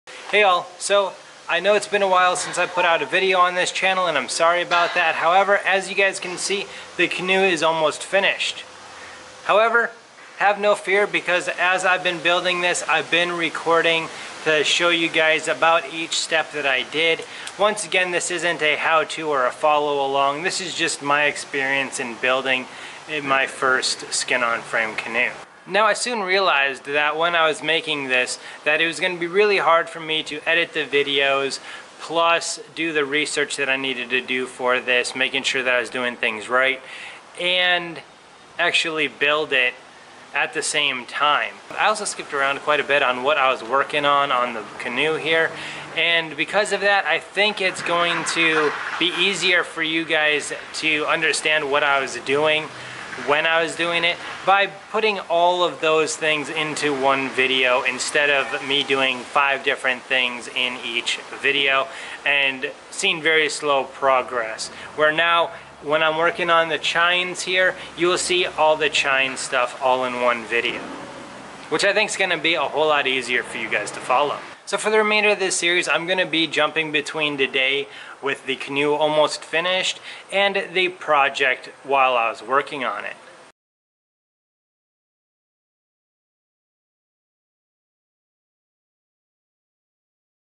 Canoe Build update - Skin on frame Canoe build
Hey just wanted to give you all a Canoe build update and let you know that I am going to be putting out my build videos of the skin on frame fuselage frame canoe soon. Sorry, it has taken me so long to post another video here hope you all like what's to come.

Support Us: